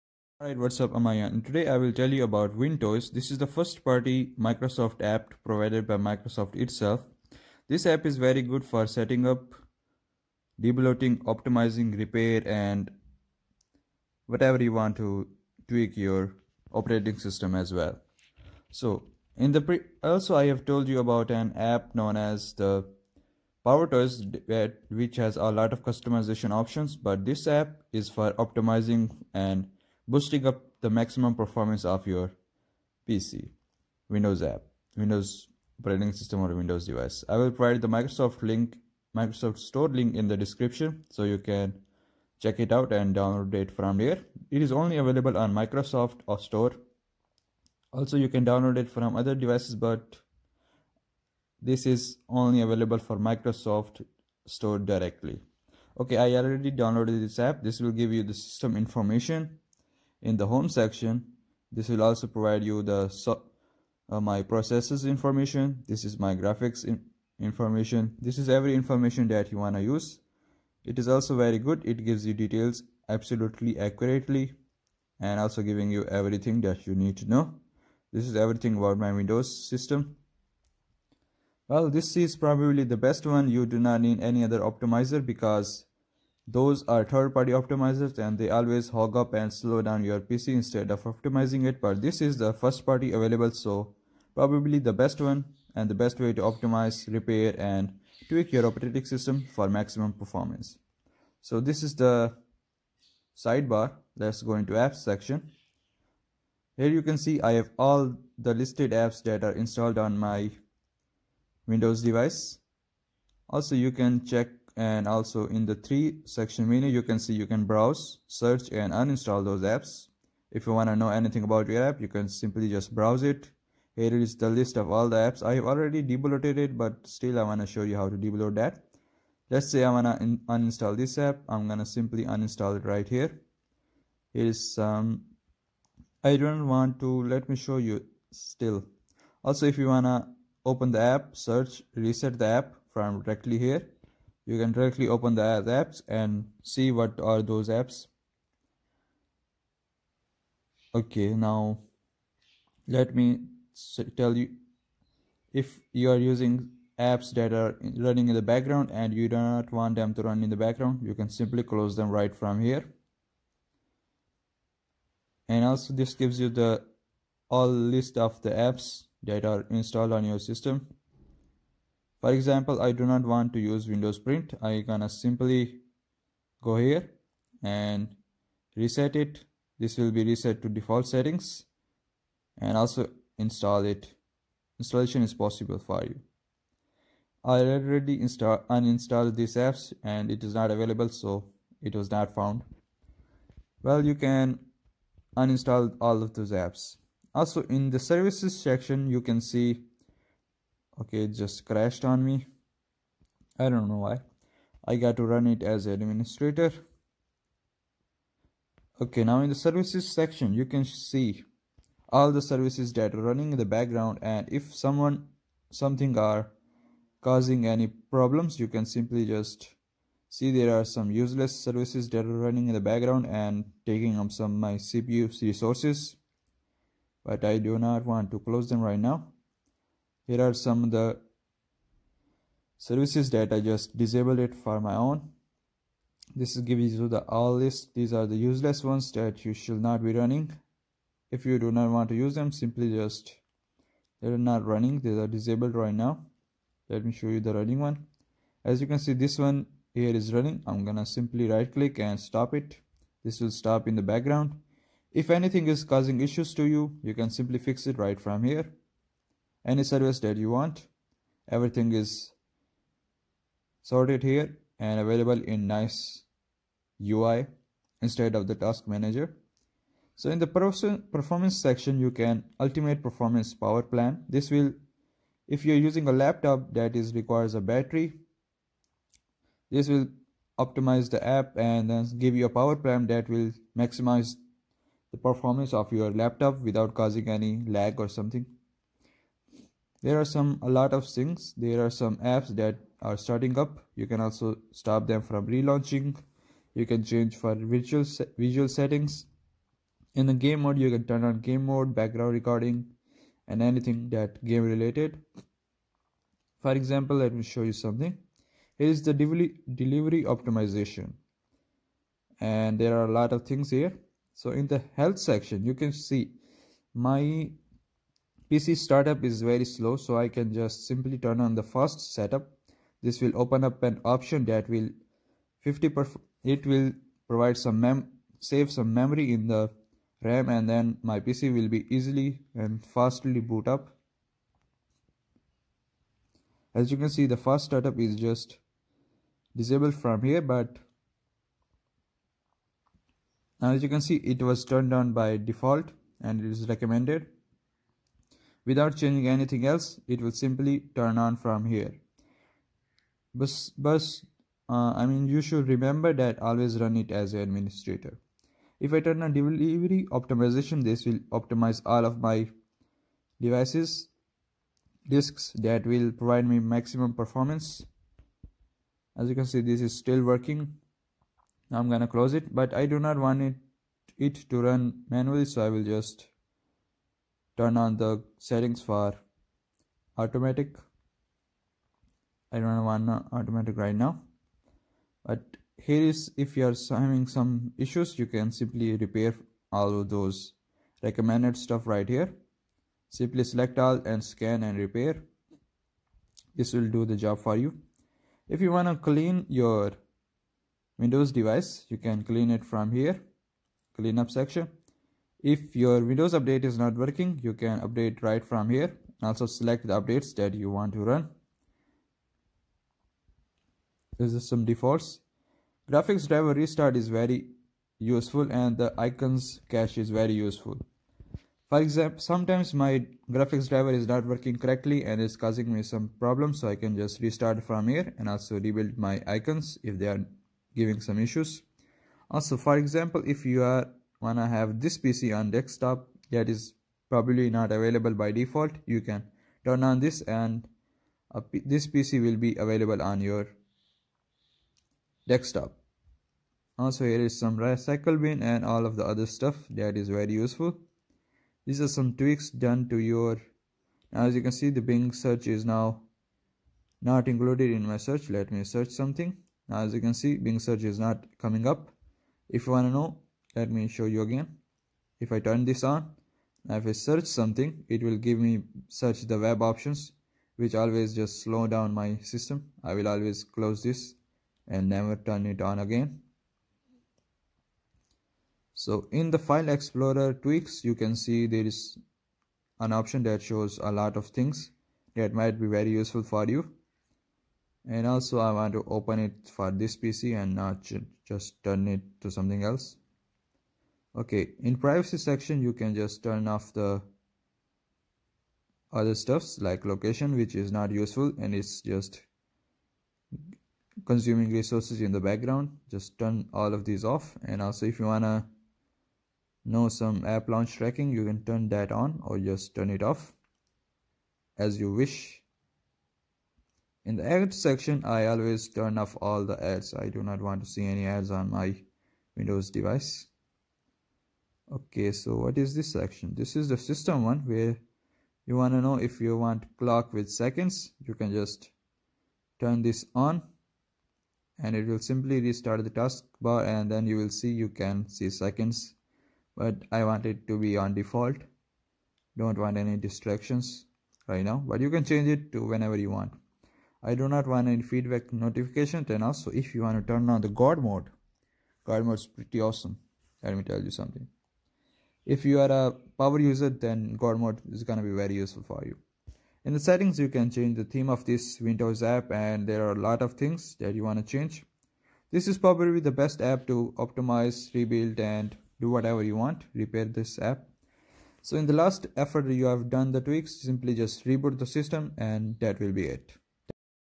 WINTOYS: THE SIMPLE CLEAN FIRST PARTY SYSTEM OPTIMIZER APP FOR MAXIMUM PERFORMANCE ON WINDOWS DEVICE
ALRIGHT, TODAY WE HAVE THIS APP CALLED 'WINTOYS' AVAILABLE ON MICROSOFT STORE. THIS PROVIDES A LOT OF USEFUL FEAUTURES LIKE "Experience Windows in your way and keep it fresh every day. Set up, debloat, optimize, repair, and tweak your operating system in a simple, time-saving, yet safe approach. Maintain it in a clean, healthy and productive state while having everything you need in one place."
PROBABLY, THE BEST ONE. ENJOY!

LINKS:

WINTOYS [ MICROSOFT STORE ]

HOW TO SETUP MICROSOFT POWERTOYS ON YOUR PC FOR A BETTER WINDOWS EXPERIENCE